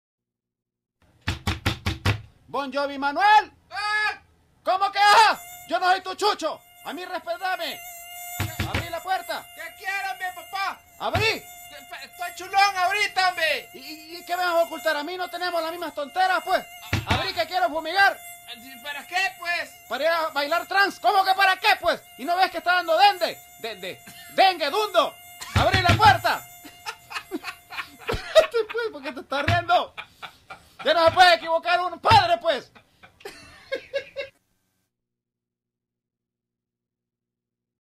Abrí la puerta! "Bon Jovi Manuel" / Radio Vox fm El Salvador
Abrí la puerta . . . "Bon Jovi Manuel"
Cuña de radio Vox FM 94.5,
Voz: Ricardo Hernández @ChiricutoVox - El Salvador